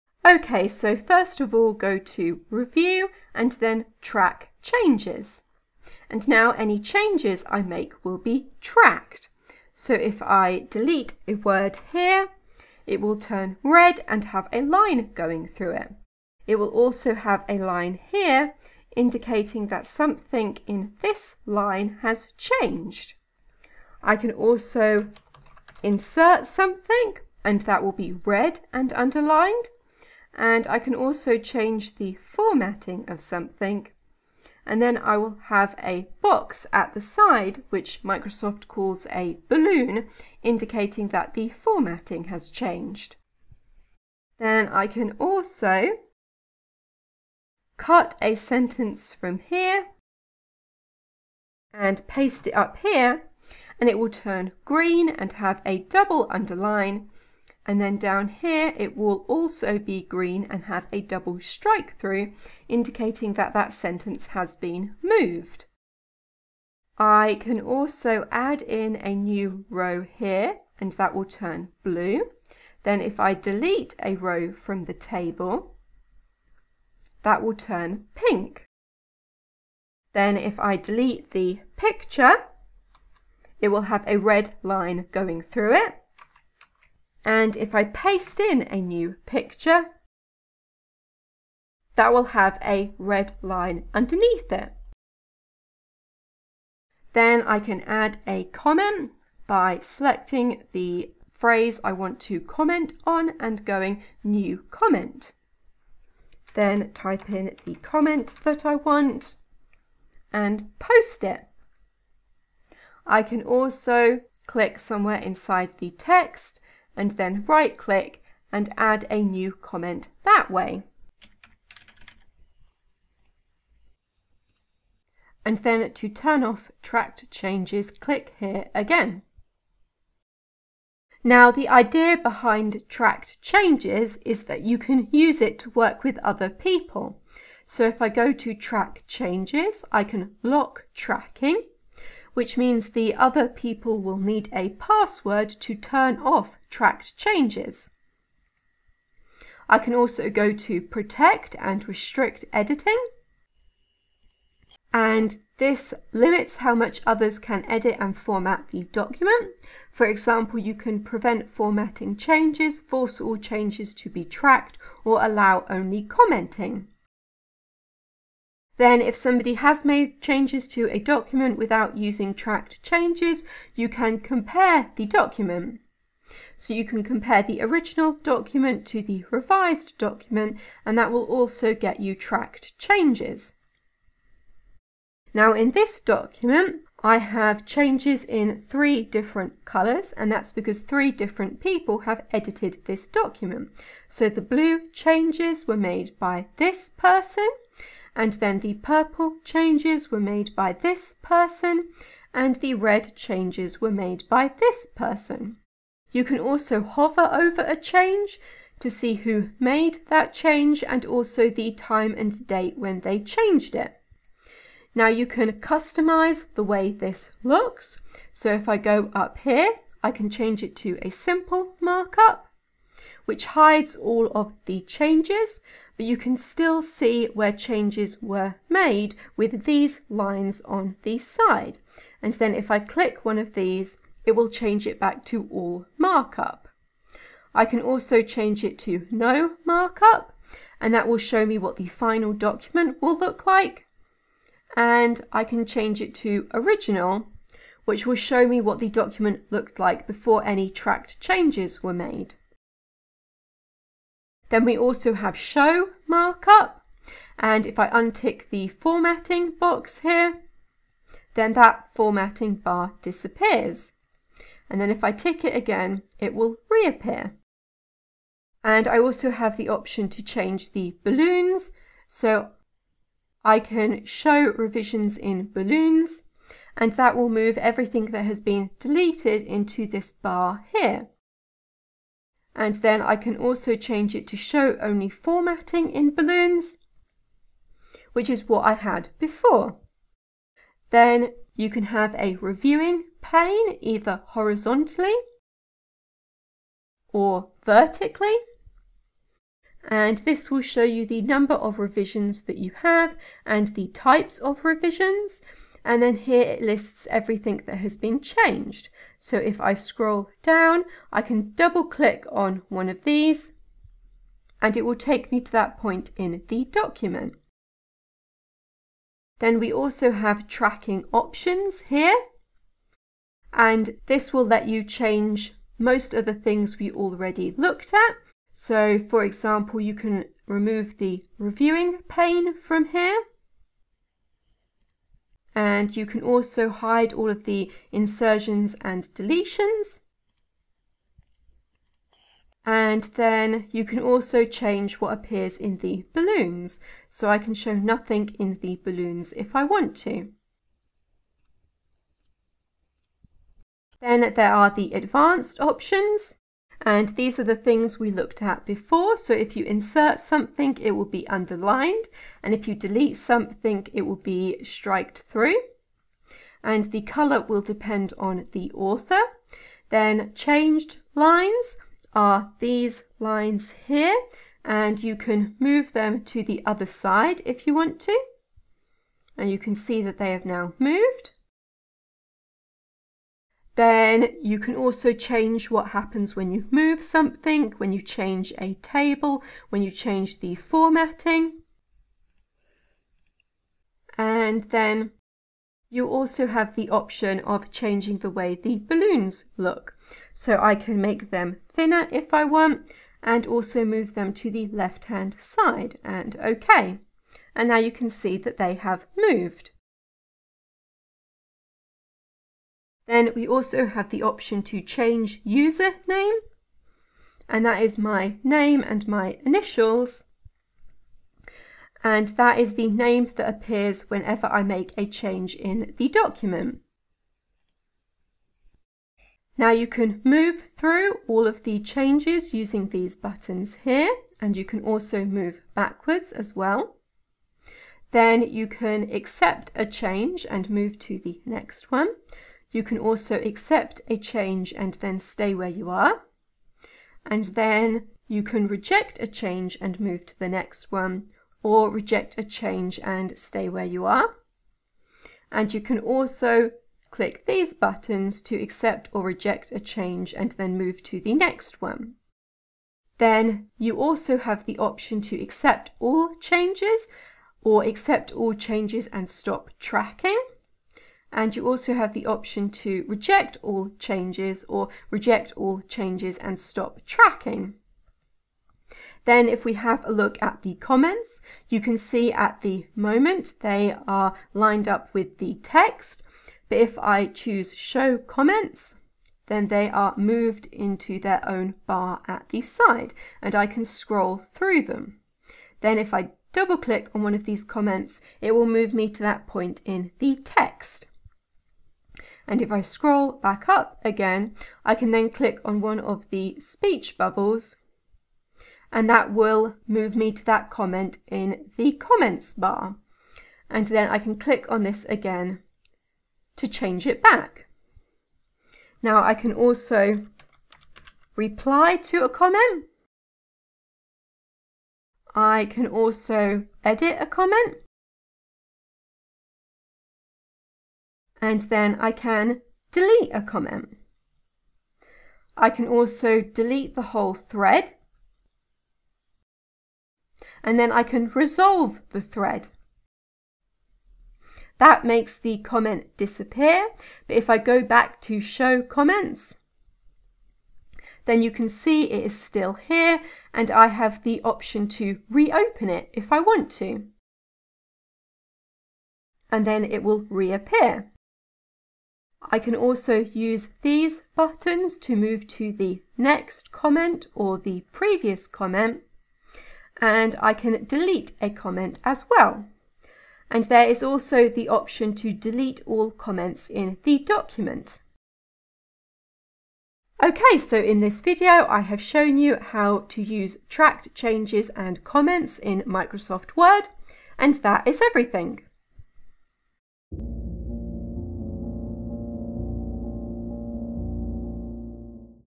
How to use comments and track changes in Microsoft Word
In the Review Tab is the option to track changes made to a document and add comments in-order to work with others.

Microsoft Office 365, 2016, Office Insider, Version 2007